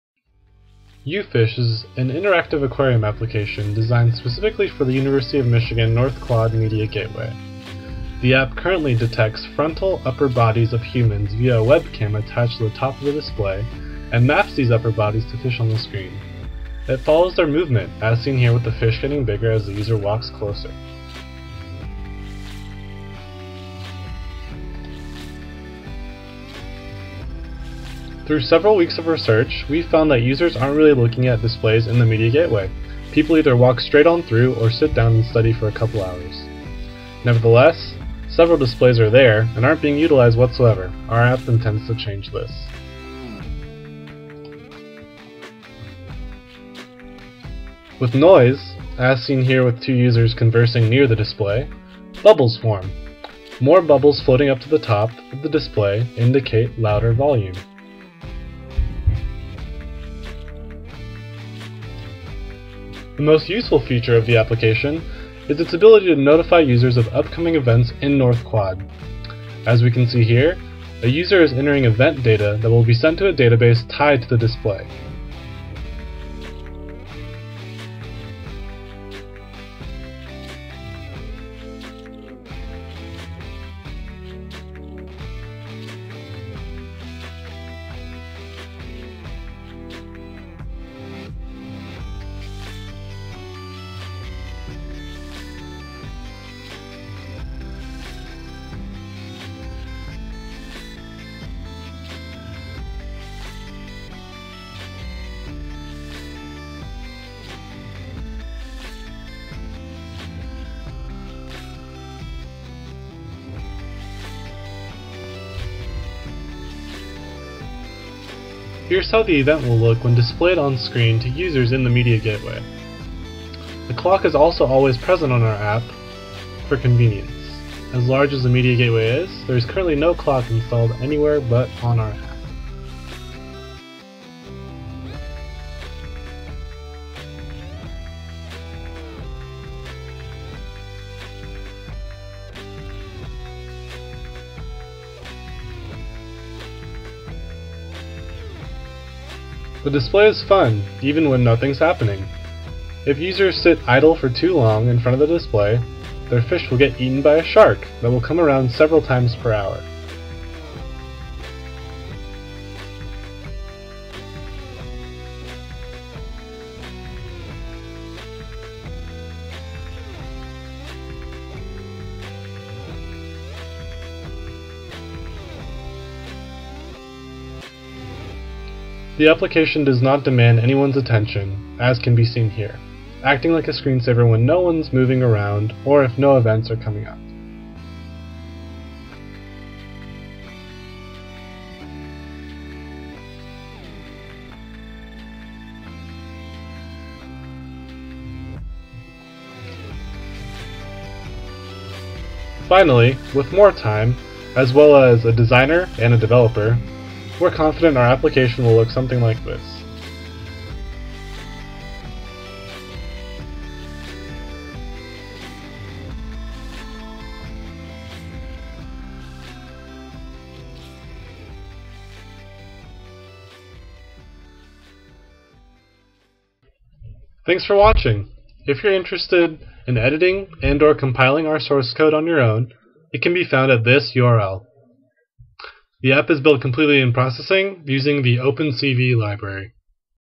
U Fish - interactive aquarium using OpenCV
Final video for a project I did this term. It's an aquarium that comes to life when it sees people! It turns people into fish! Fish movement follow people movement! Bubbles = noise! Event notifications! AHHH! So awesome!